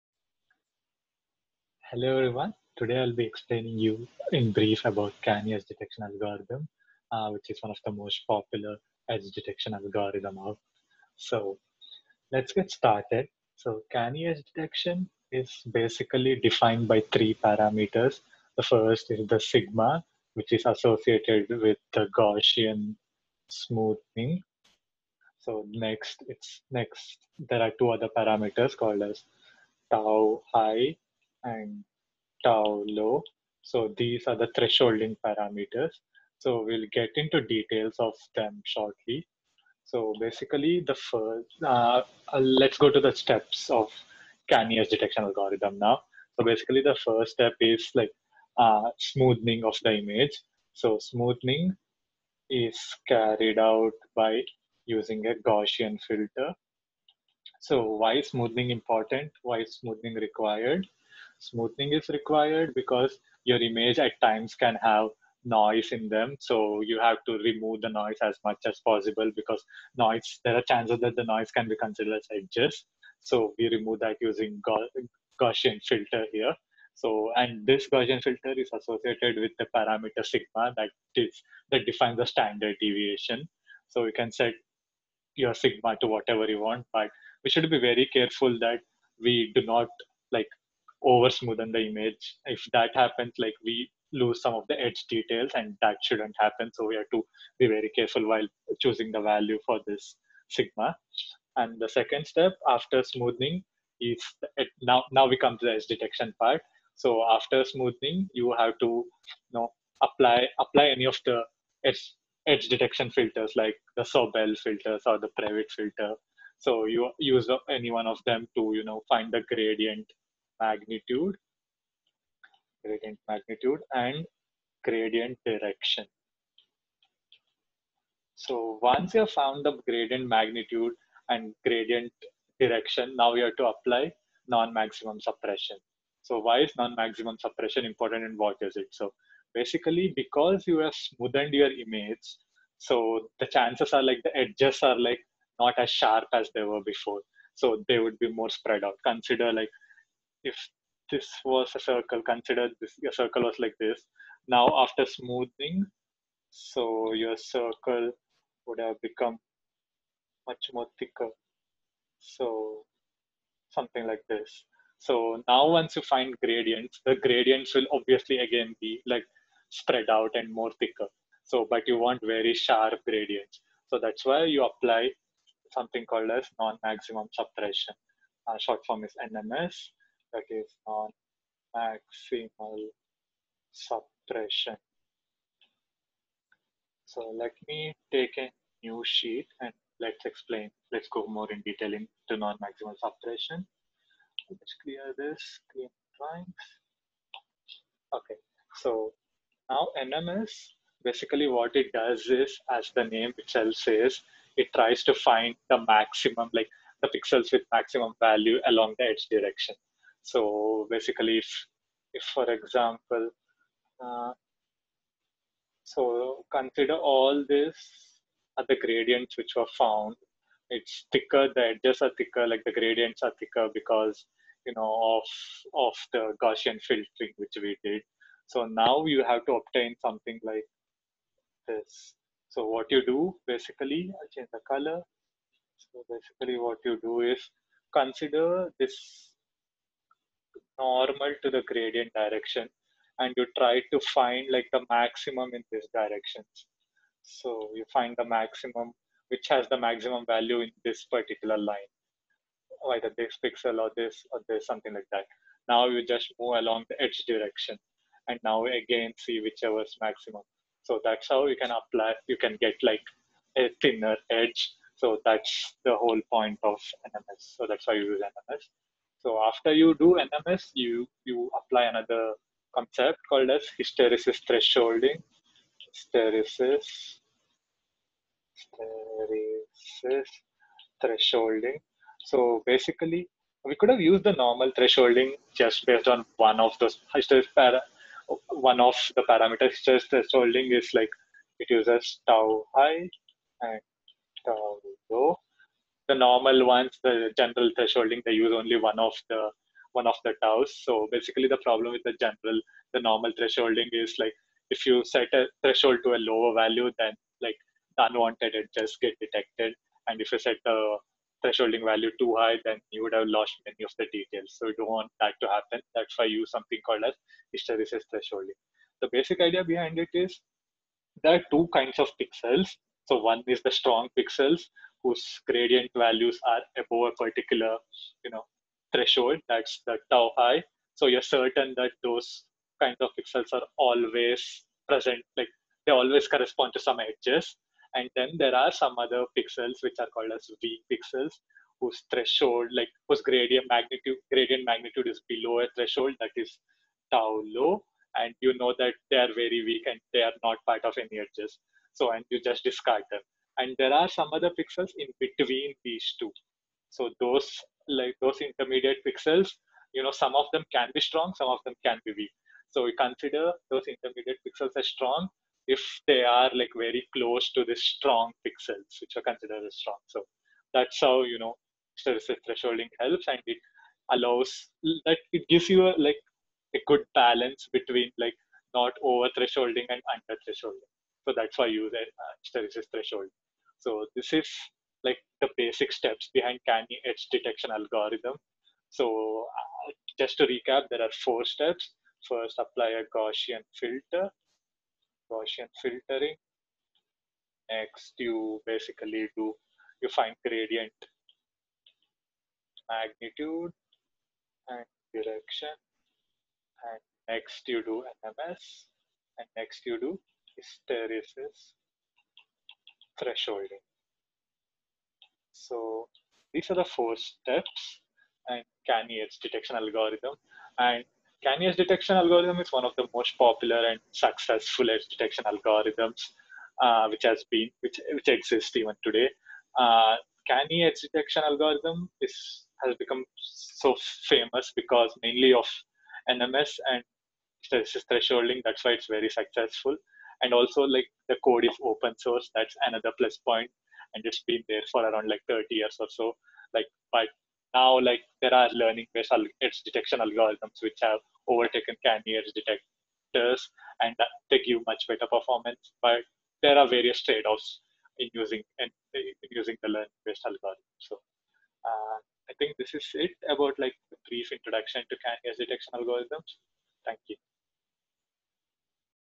Introduction to Canny Edge Detection algorithm
This video gives a brief idea about Canny Edge Detection and why it is popular